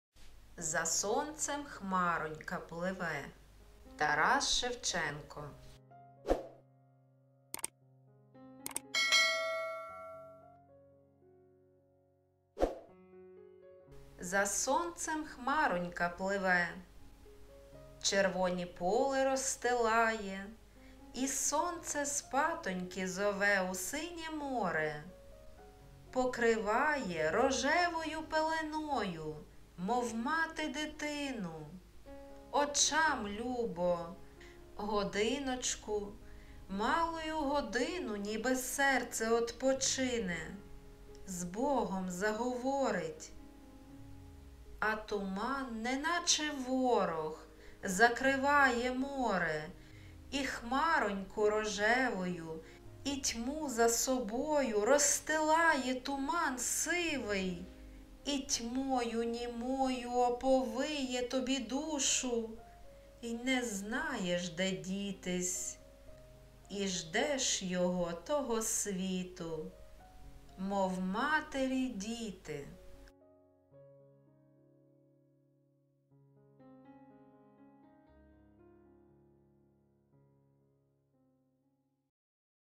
За сонцем хмаронька пливе... – Тарас Шевченко
Пропоную для вашої уваги це відео із поетичним твором українського автора Тараса Шевченка "За сонцем хмаронька пливе". Бажаю приємного перегляду та задоволення від прослуховування.

За сонцем хмаронька пливе,
червоні поли розстилає
і сонце спатоньки зове
у синє море: покриває
рожевою пеленою,
мов мати дитину.
Очам любо. Годиночку,
малую годину
ніби серце одпочине,
з Богом заговорить...
А туман, неначе ворог,
закриває море
і хмароньку рожевую,
і тьму за собою
розстилає туман сивий,
і тьмою німою
оповиє тобі душу,
й не знаєш, де дітись,
і ждеш його, того світу,
мов матері діти.